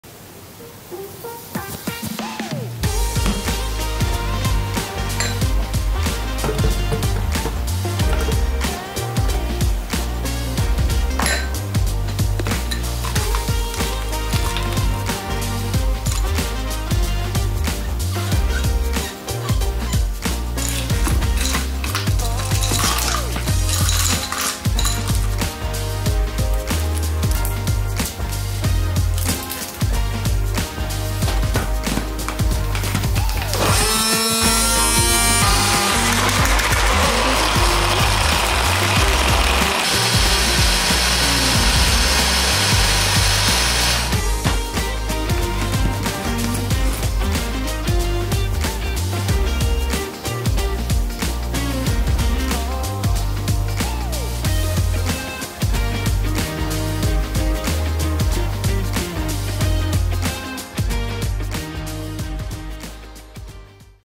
【コードレスミキサー】洋梨とミックスベリーのスムージー
秋が旬の洋梨を使ったスムージーです！

【材料】
・洋梨：180 g
・ミックスベリー：30 g
・ヨーグルト：40 g
・氷：70 g
・蜂蜜：15 g